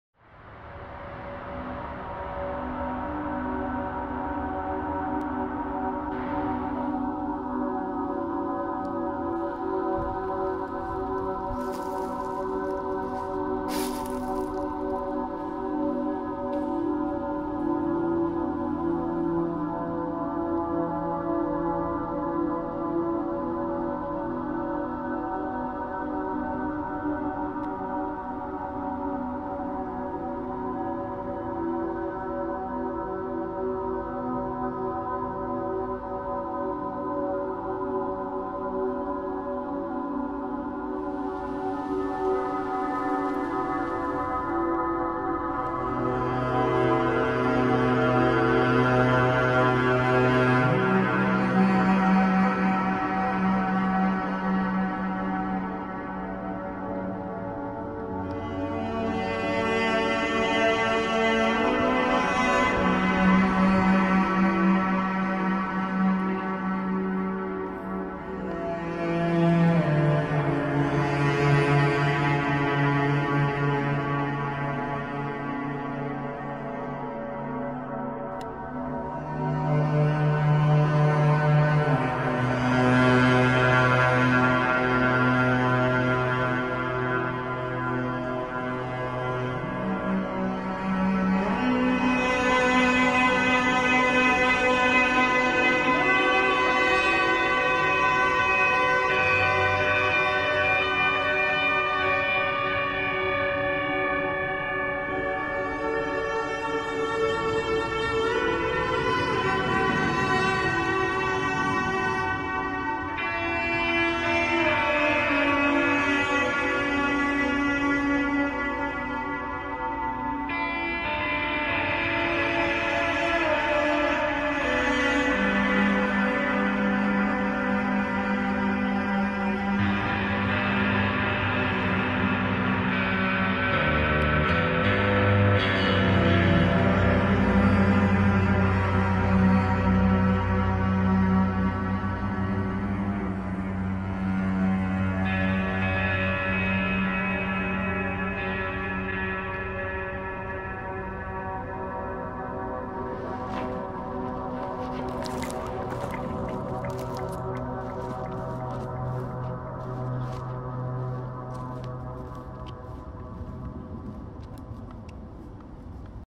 Training Area Ambiente
Eine kleine Ambiente/Cinematic von den besten Aufnahmen aus der Granatenschießstand Dokumentation ...